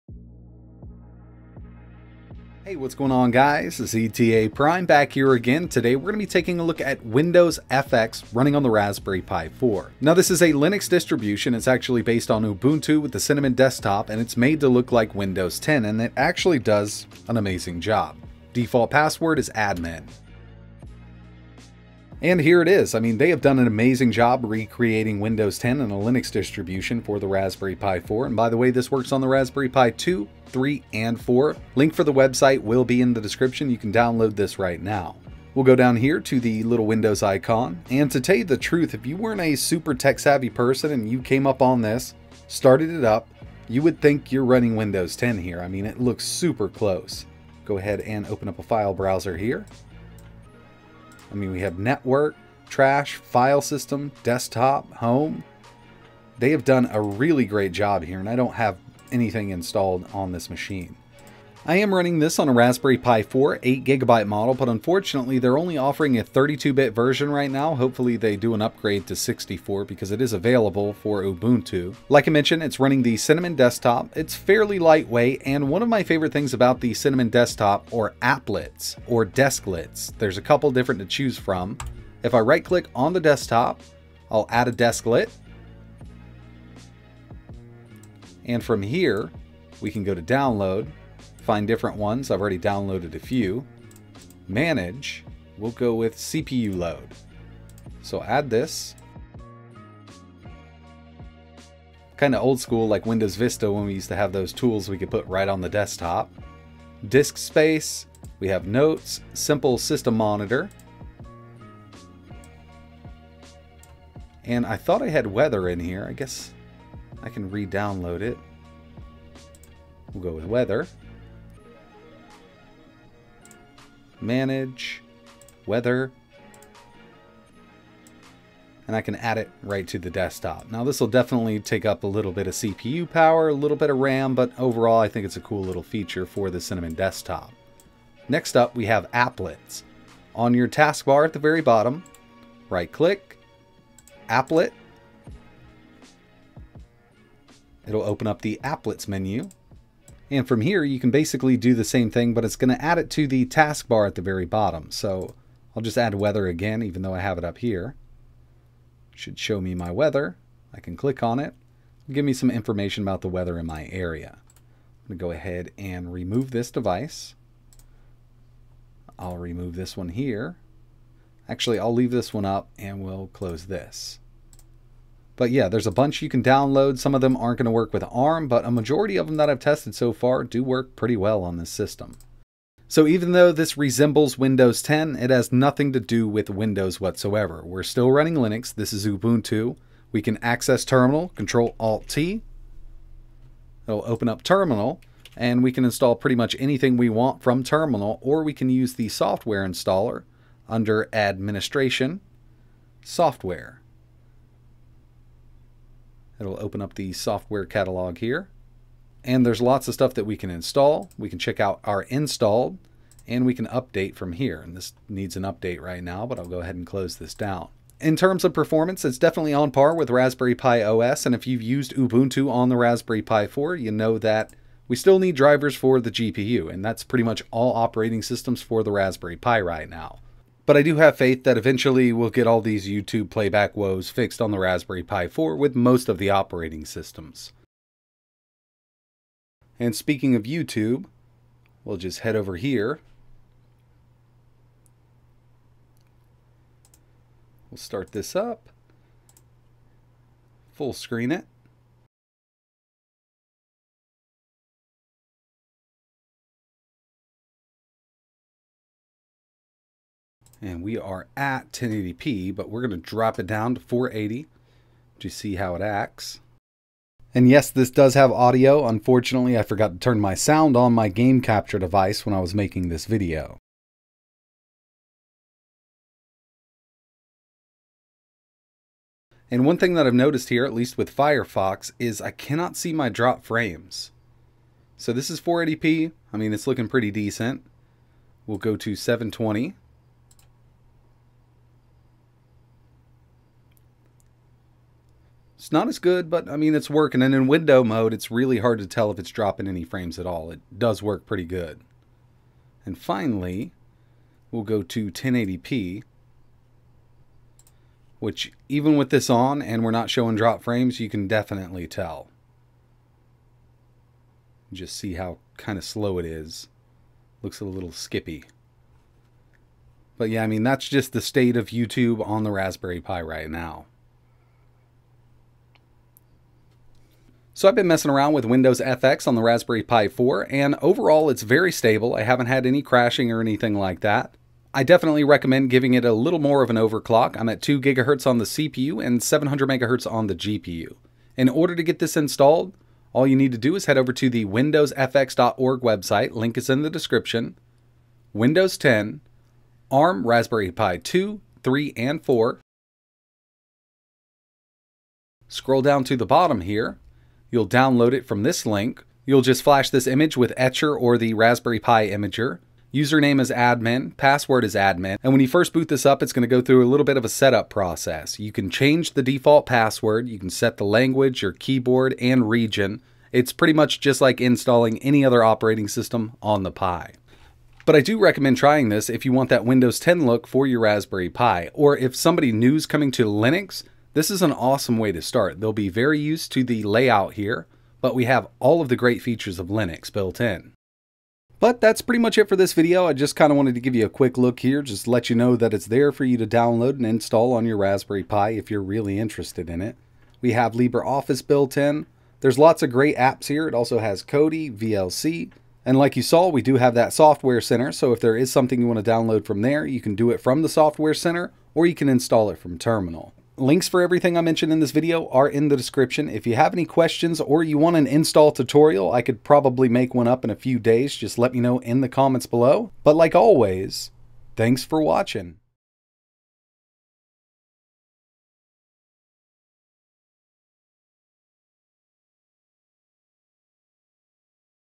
WindowsFX For the Raspberry Pi 4 Is Awesome! Ubuntu 20.04 + Cinnamon Desktop + Windows 10 Looks
In this video, we take a quick look at WindowsFX for the Raspberry Pi 4 from the creators on LinuxFX!
WIndows FX is designed to look like Windows 10 but its actually Ubuntu 20.04 with the Cinnamon desktop!
If you want that Windows 10 lool on your Raspberry Pi  I highly recommend trying Windows FX out.

Pi4 4GB
Micro Sd Card
Power supply
Micro HDMI